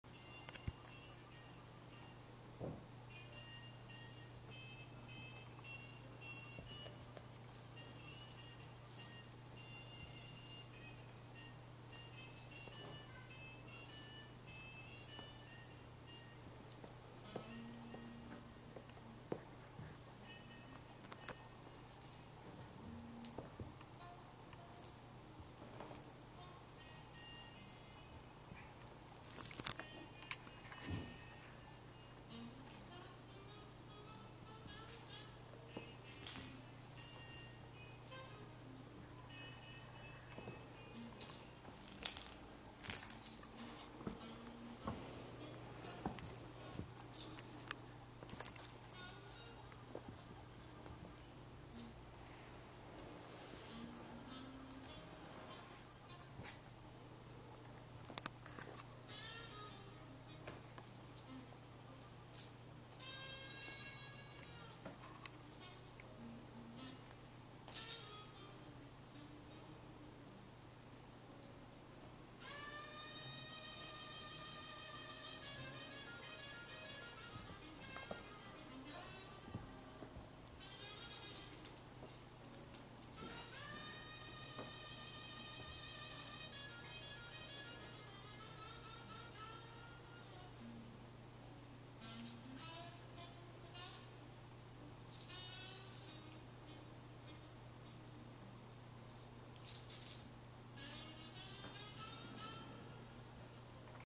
Ajrams kitten's playing with their new set
The little lions playing with their new toy set, they love it.